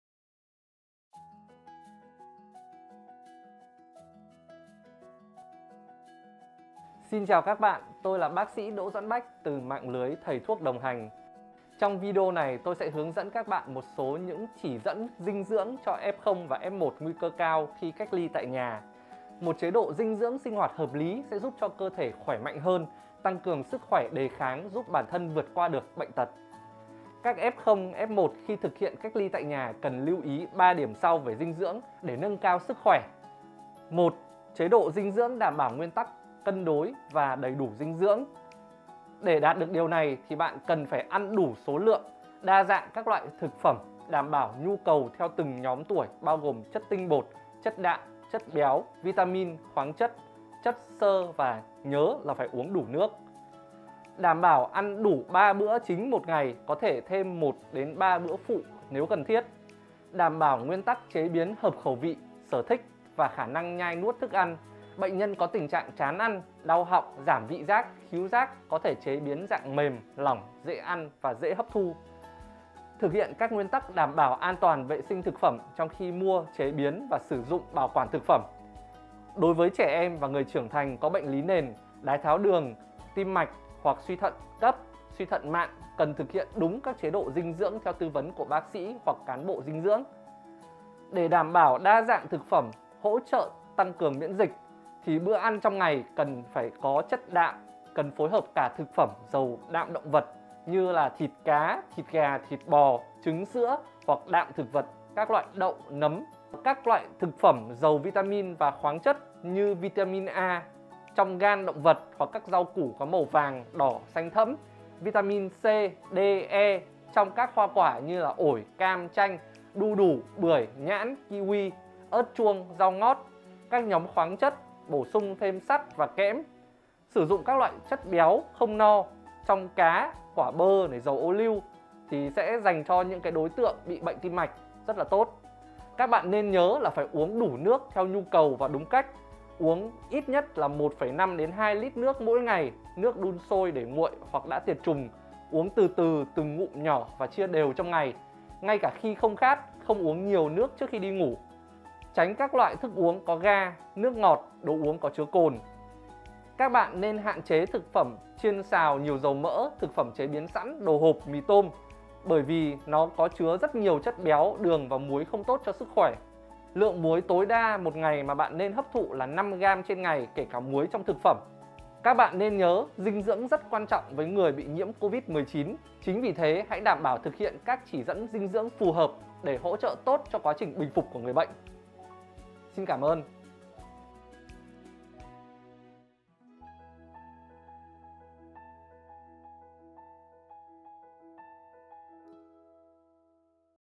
Dinh dưỡng cho người Covid
Chế độ ăn cho người mắc Covid-19 góp phần quan trọng giúp mau bình phục, đẩy lùi triệu chứng, ngăn chặn biến chứng nặng.